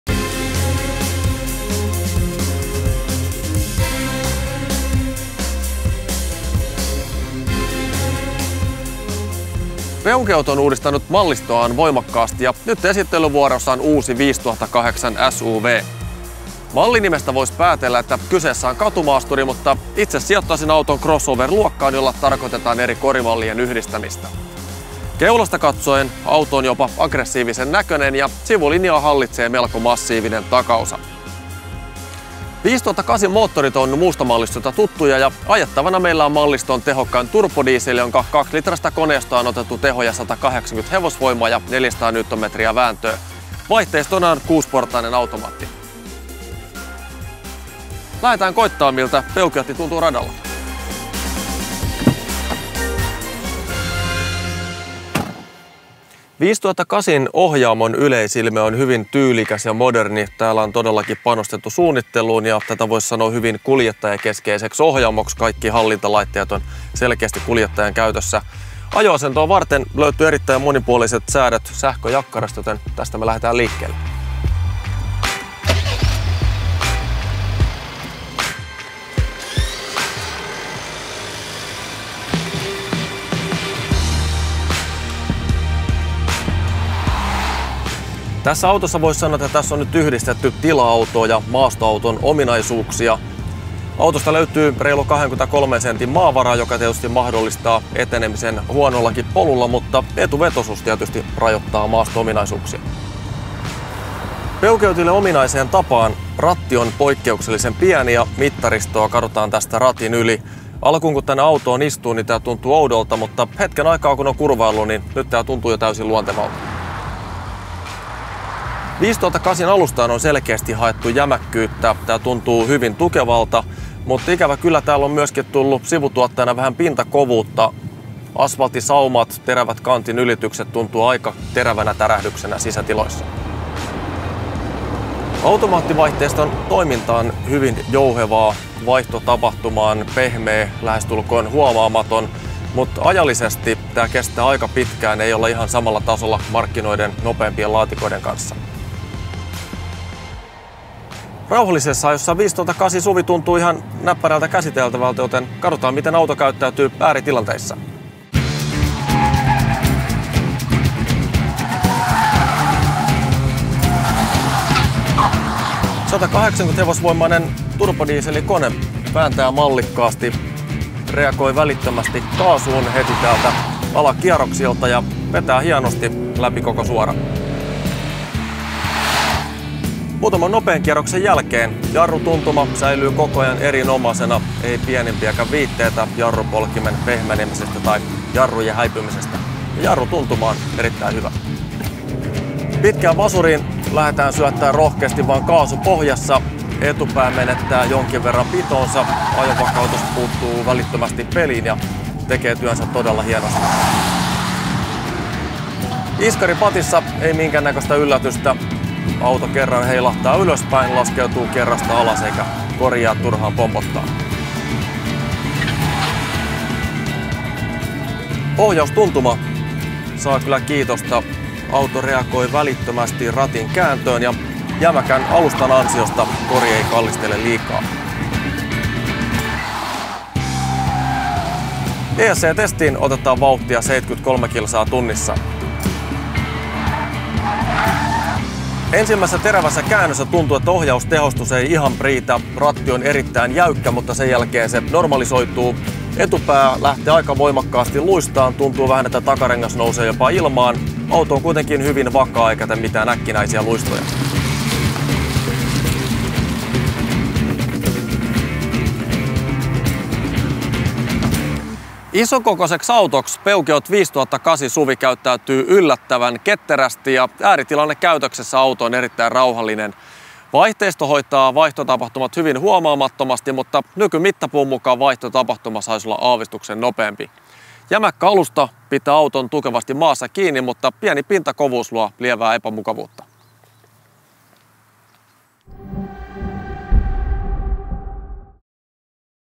Testissä uusi Peugeot 5008 (Teknavi 2017)
Peugeot 5008 muuttui tila-autosta katumaasturiksi. Risto Virtasen selvittää, miten uutuus pärjää ajotestissä.